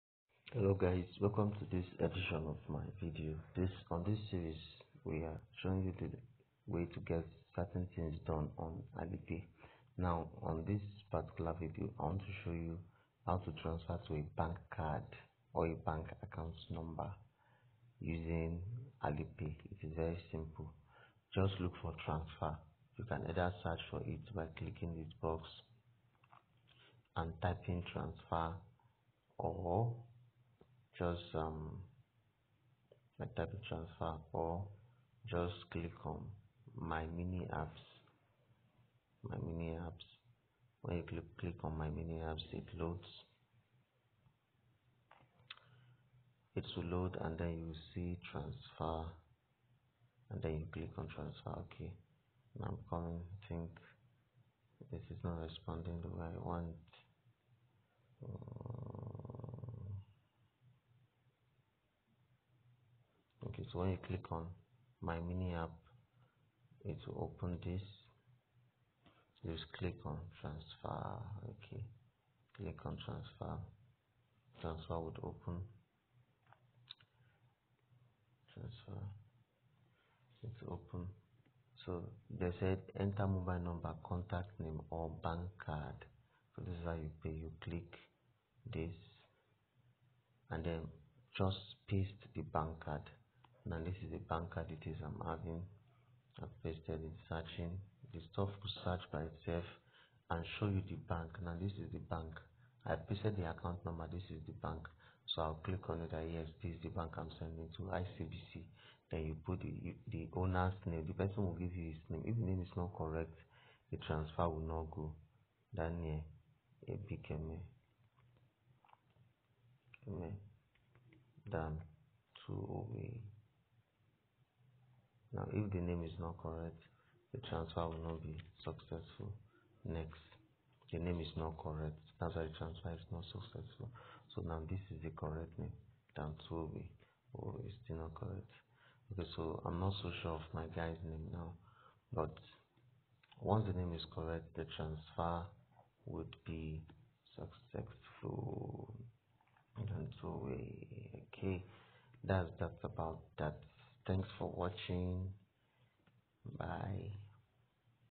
HOW TO TRANSFER FROM ALIPAY TO BANK
Do you want to make payments from your Alipay account to a Chinese bank card? here's a step by step guide